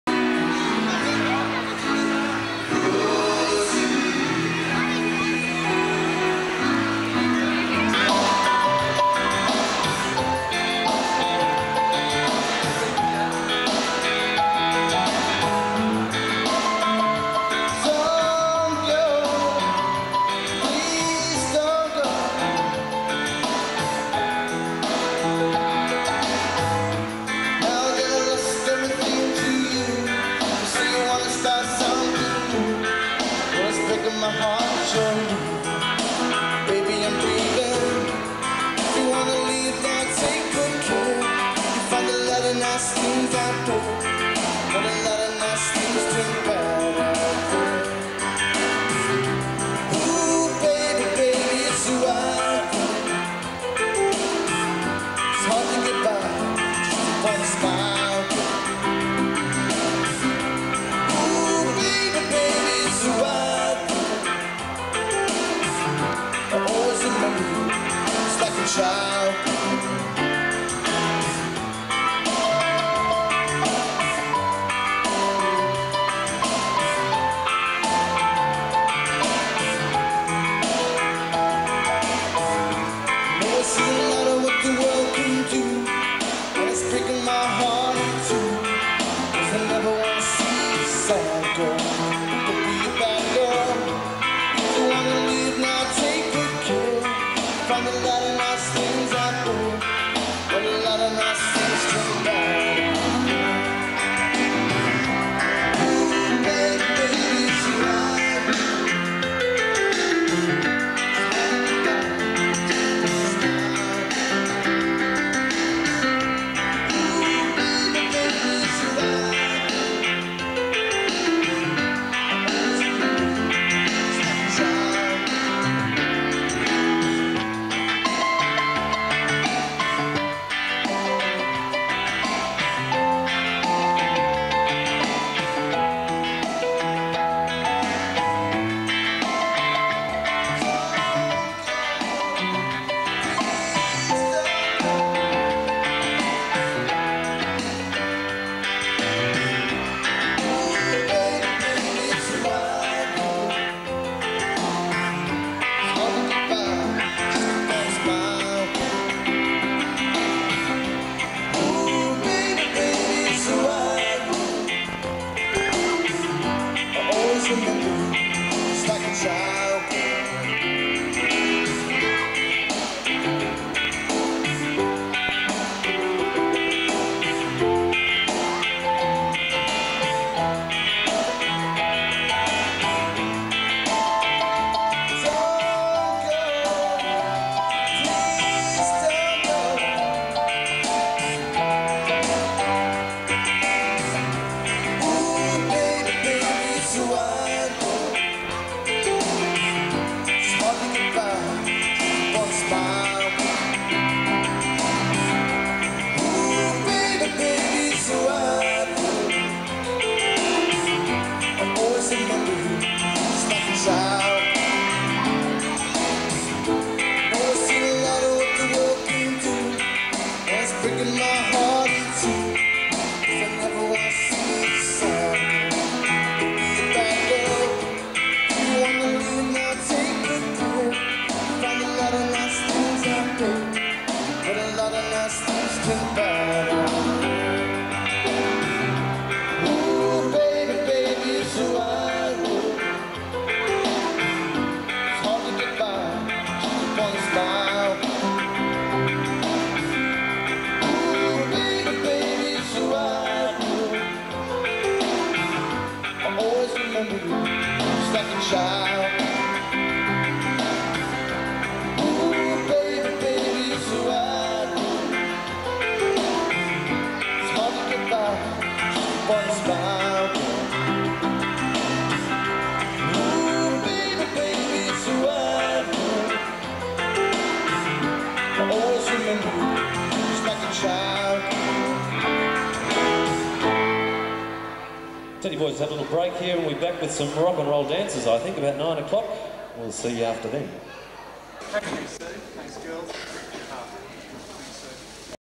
TEDDY BOYS PICNIC - Musical Trio
Teddy Boys Picnic - Recorded Live at Wangaratta Town Hall Cup Eve 1988.

Teddy Boys Picnic were a successful Trio of musicians living and working in North East Victoria, Australia in the late 80's to early 90's playing covers and a couple of original tunes.

Teddy Boys Picnic were: Mike Bernard - Backing Vocals,  Keyboards/Bass and Drum Machine, Lyndsey Steer on Lead/Backing Vocals and Lead Guitar and John Delves on Lead Vocals and Electric/Acoustic Guitars.